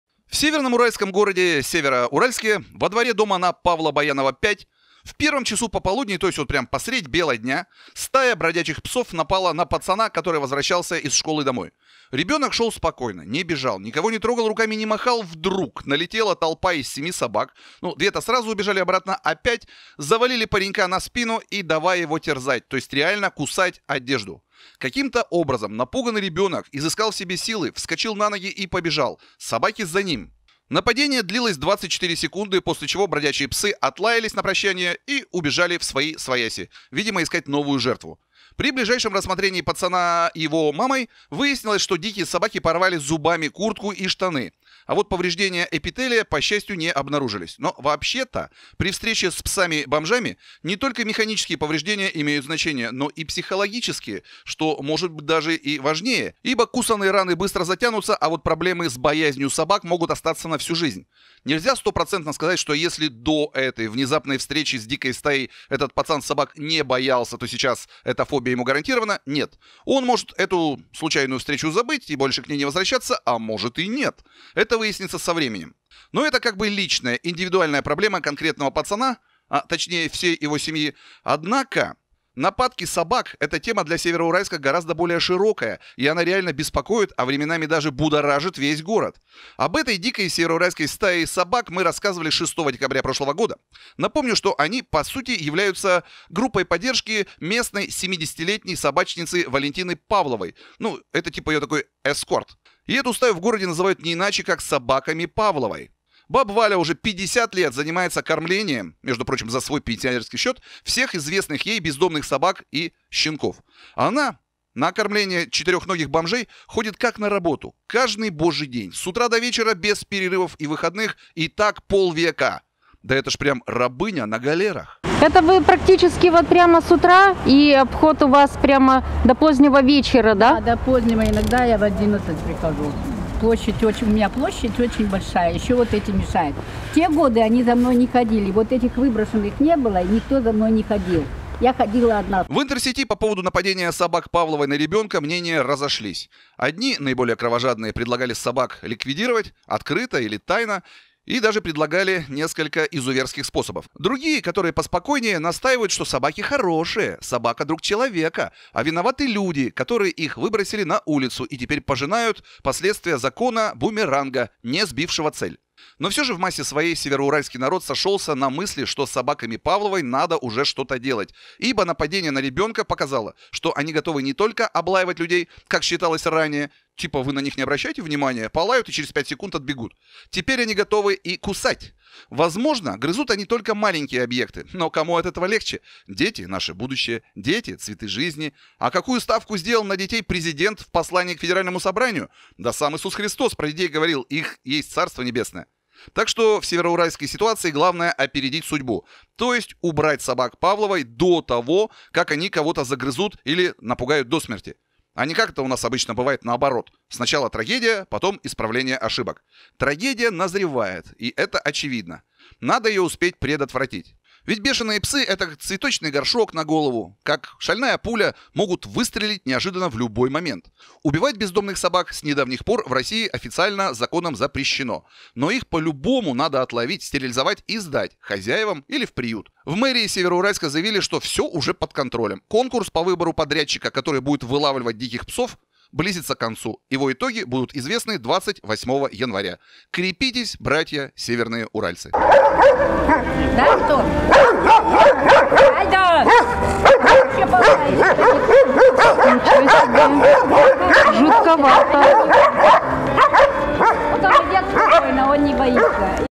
5 бездомных псов грызут лежащего ребенка. Real Video
Кто остановит нападения на людей собак Павловой в Североуральске?